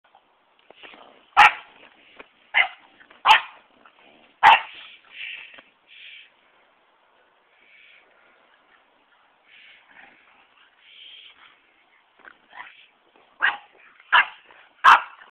Playing with Monique 2
Our French bulldog goes goofy and tackles our english bulldog